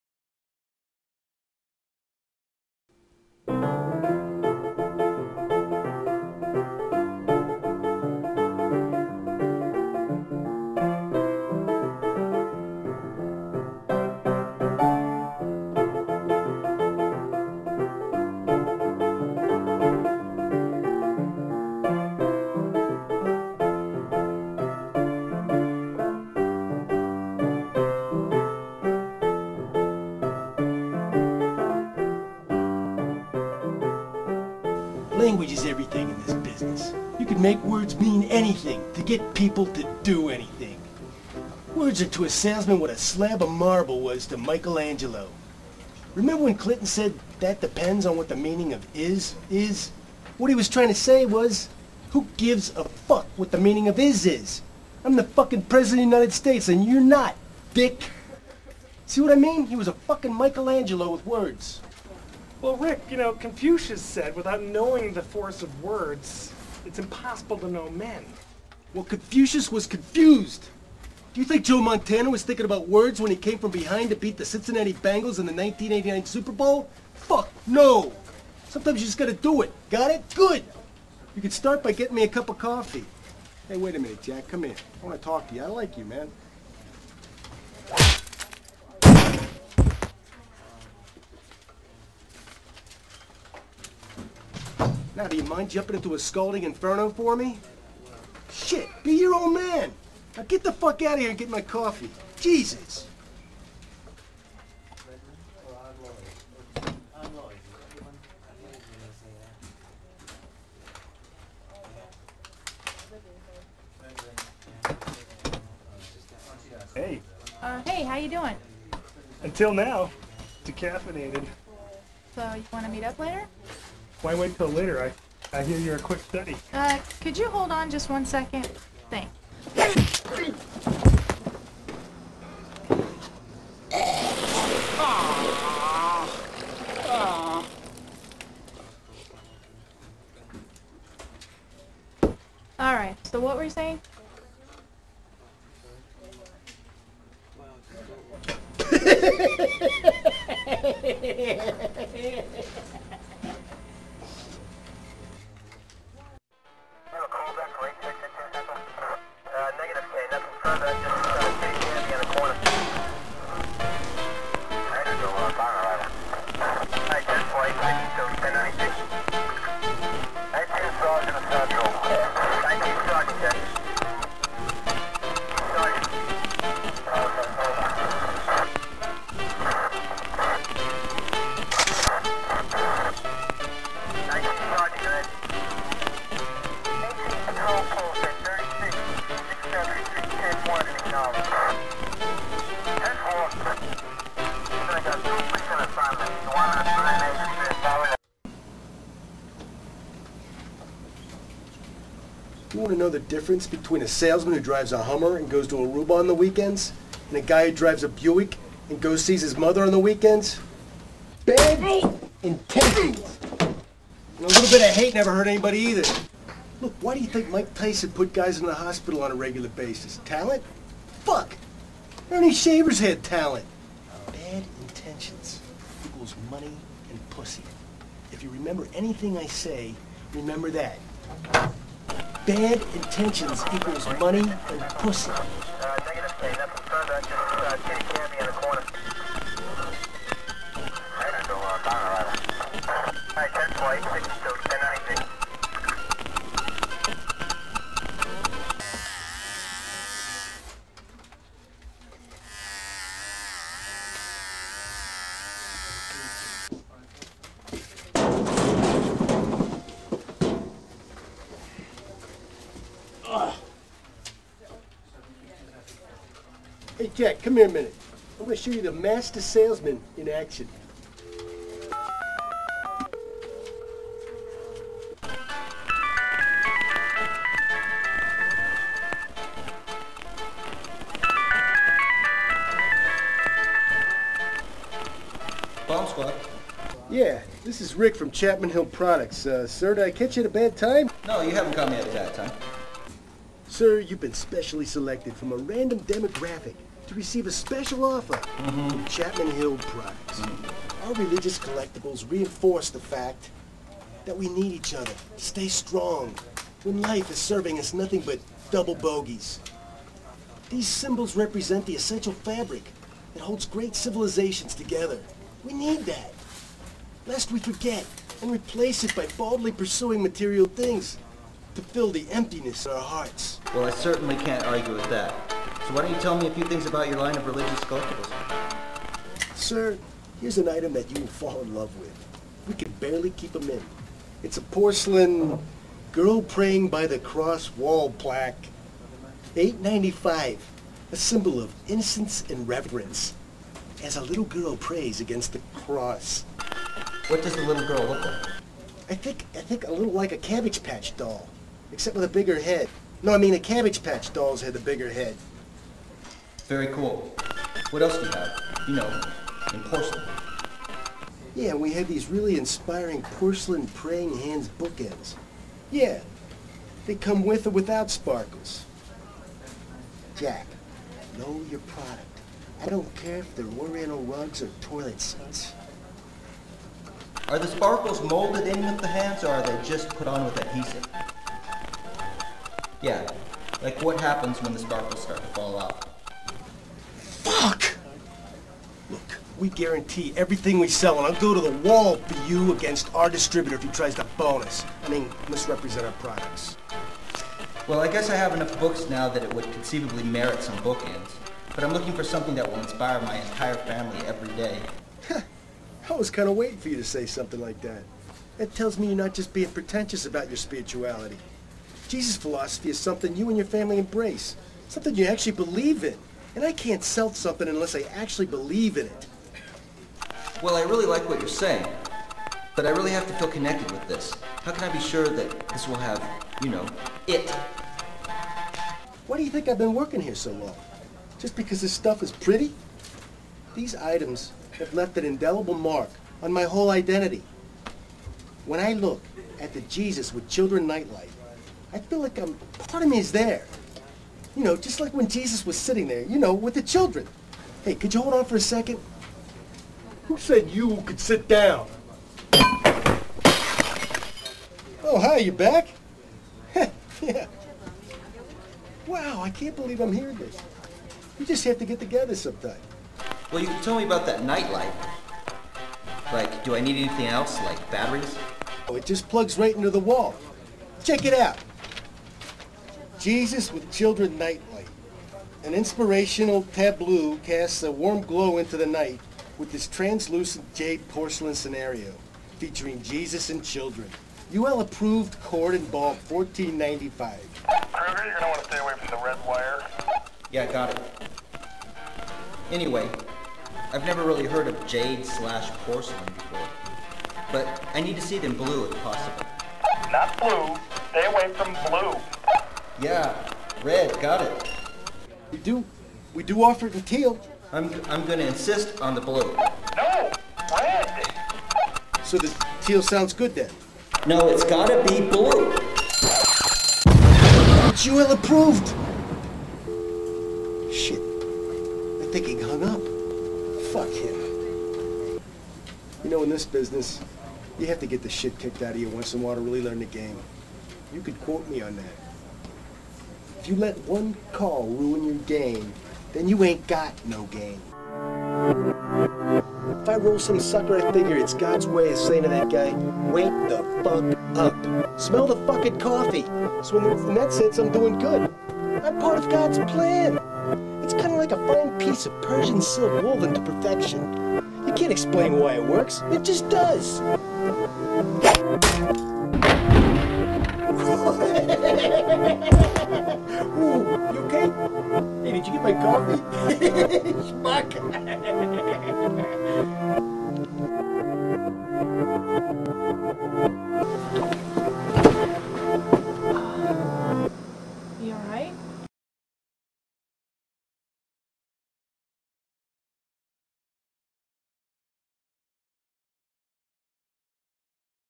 Bad Intentions
A film made for the NYC Midnight Movie Making Madness Competition 2003. The assignment: A slapstick comedy about a telemarketing company.
The Dog Valley Dirtballs got 1st place in their heat and made the trip to NYC to make a film in 24 hours. Ultimately, they failed miserably. Oh, well.